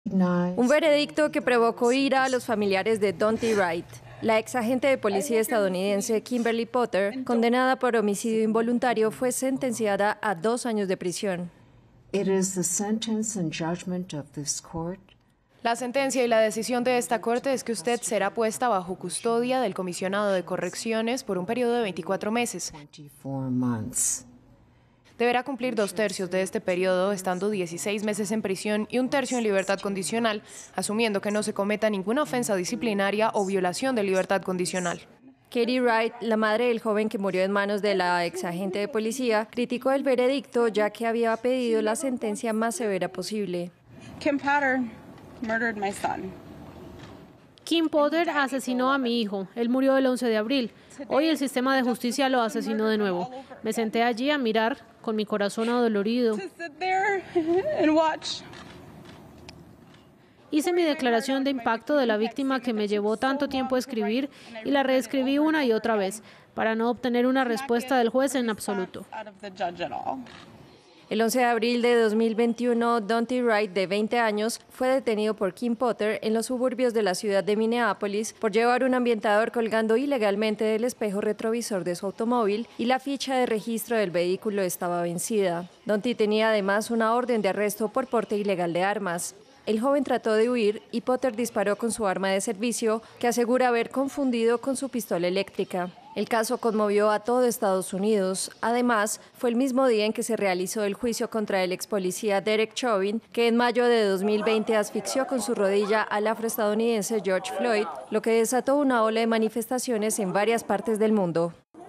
Expolicía Kimberly Potter fue condenada a dos años de prisión por la muerte de Daunte Wright
El incidente se produjo durante un control de tráfico rutinario en los suburbios de Minneapolis. Mientras Kimberly Potter y otros agentes intentaban detener a Wright por una orden de captura pendiente por posesión de armas, el afroamericano de 20 años se resistió al arresto. La expolicía disparó un solo tiro en el pecho, hiriéndolo fatalmente. Kimberly Potter "cometió un trágico error" al realizar "una detención legal", explicó la jueza Regina Chu, para justificar la sentencia indulgente.